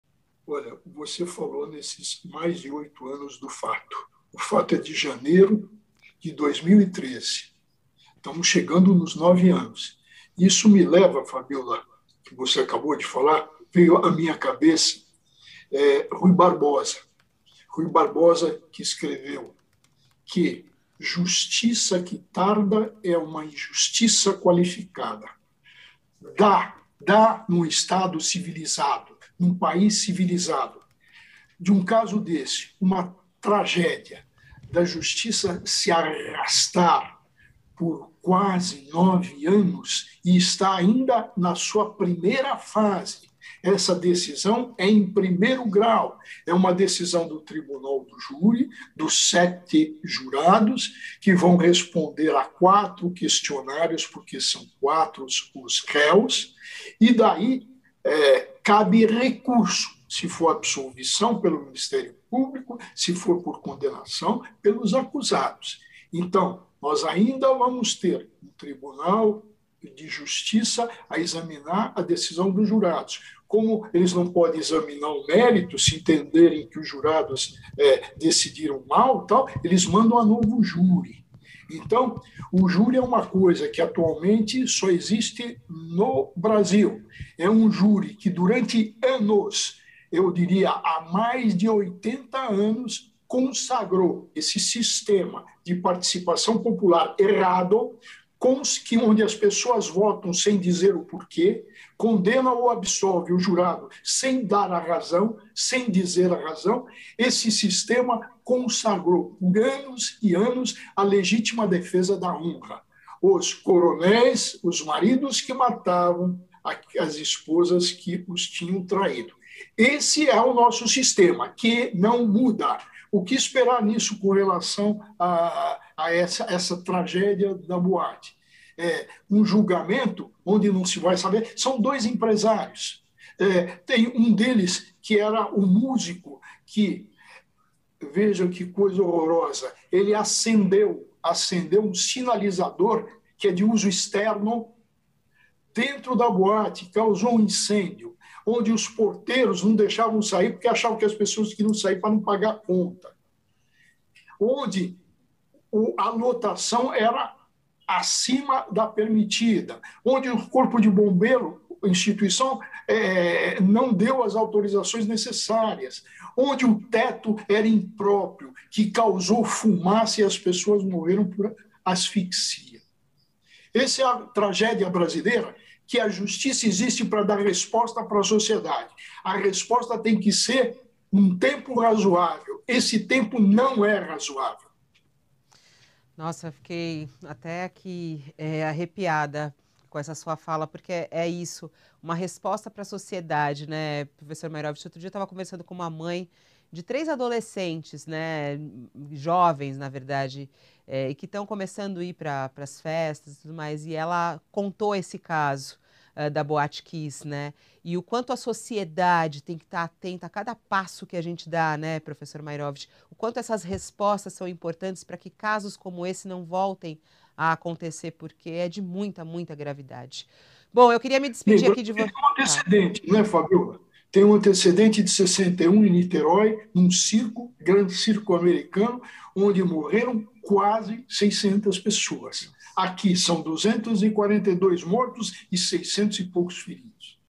Caso Boate Kiss: 'Justiça deve uma resposta à sociedade em tempo razoável', diz jurista
No UOL News, o jurista e colunista do UOL Walter Maieróvitch comentou o julgamento dos acusados pelo incêndio da Boate Kiss em Santa Maria (RS), em 2013, que deixou 242 mortos e mais de 600 feridos.